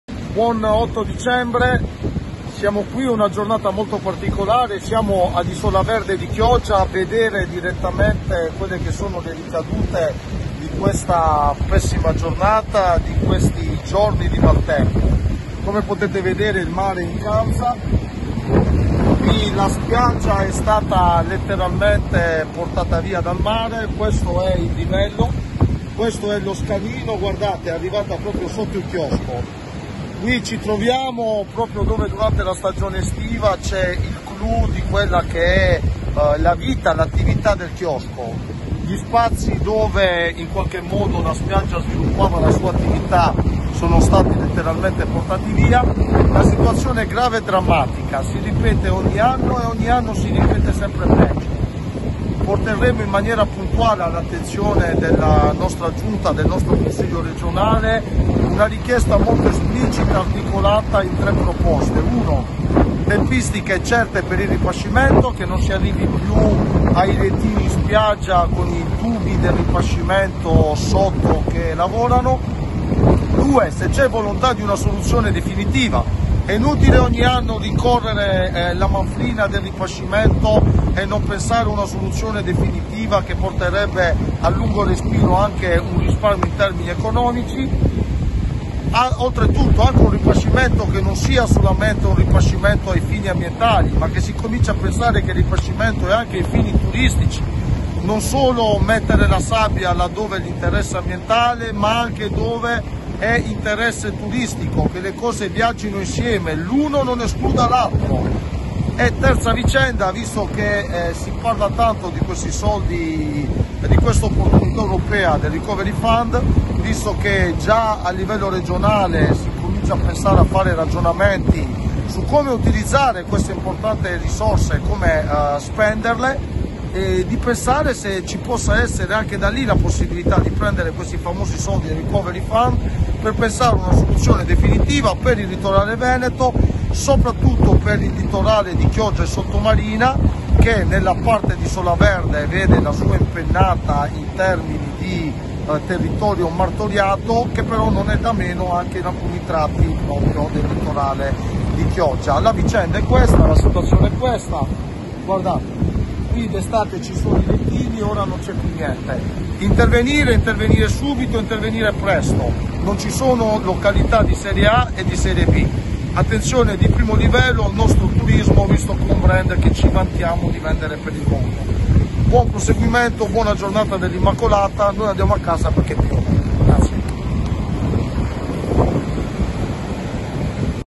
MAREGGIATE A ISOLAVERDE, MONTANARIELLO: «UTILIZZARE IL RECOVERY FUND EUROPEO PER AZIONI STRUTTURALI»
La politica tiene accesa l'attenzione attorno alle mareggiate che nello scorso weekend si sono abbattute sul litorale di Sottomarina e Isolaverde. Il consigliere regionale Jonatan Montanariello è stato in sopralluogo nelle zone colpite: «La situazione è gravissima, drammatica. L’acqua ha travolto tutto». L'esponente dell'opposizione intende comprendere quali siano i progetti per la difesa del litorale, i tempi e le modalità del ripascimento e della manutenzione delle spiagge: «Non è chiaro - continua Montanariello - se riguarderà esclusivamente la tutela ambientale o anche le attività economiche. Dobbiamo incrementare le risorse regionali, ma è evidente che non possono bastare per risolvere il problema della difesa della costa».

Per questo il consigliere vedrebbe con favore l'eventuale utilizzo del Recovery Fund europeo: «È un’opportunità imperdibile. Visto che Zaia ha già fatto la "lista della spesa", chiedendo 25 miliardi per finanziare una serie di progetti - prosegue l'amministratore chioggiotto - crediamo che una partita del genere non possa essere esclusa. La Regione si confronti con il territorio e presenti un piano dettagliato per ottenere i fondi necessari a proteggere il litorale. È impensabile trovarsi ogni anno di fronte alla stessa devastazione, urgono un piano serio e finanziamenti adeguati». Il vicepresidente della commissione Politiche del Territorio e Ambiente annuncia anche un'interrogazione urgente, e la presentazione di emendamenti alla prossima sessione di bilancio.